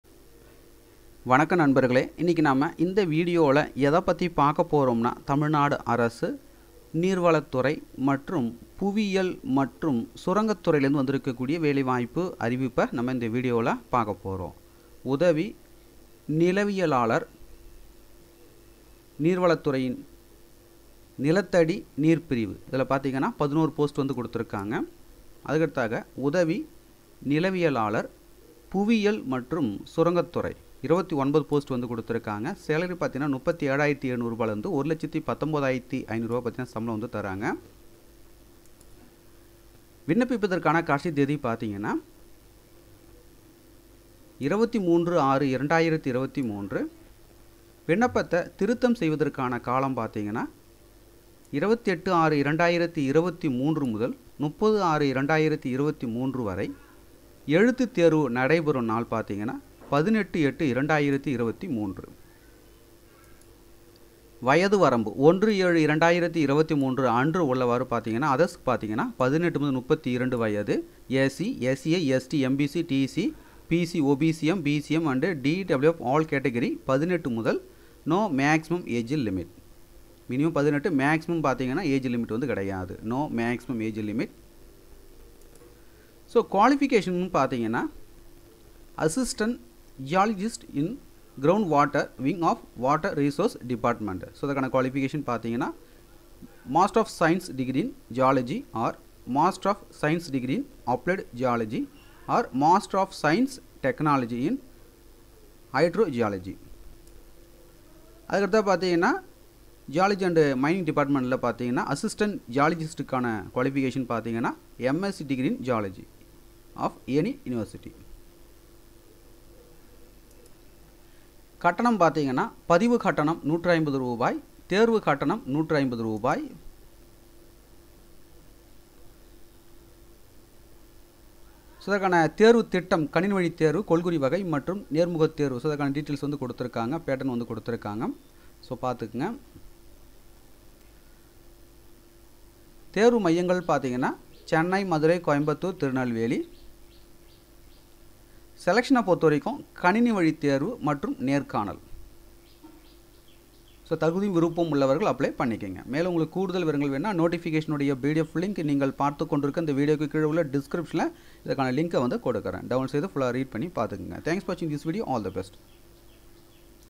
Tamilnadu Water Resources Department Recruitment 2023 | TN Geology and Mining Department 2023
Tamilnadu Ground water wing of Water Resources Department Recruitment 2023 | Tamilnadu Geology and Mining Department 2023

AGE LIMIT (as on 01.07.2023): SCs, SC(A)s, STs, MBCs/DCs, BC(OBCM)s, BCMs and Destitute
Widows of all Categories : 18 Years - No maximum Age limit

OTHERS : 18 Years -32 years
உதவி நிலவியலாளர் (நீர்வளத் துறையின் நிலத்தடி நீர் பிரிவு) & புவியியல்  மற்றும் சுரங்கத்
துறை

மேலும் முழு விபரங்கள் பெற கீழே உள்ள லிங்க்கை  கிளிக் செய்யுங்கள்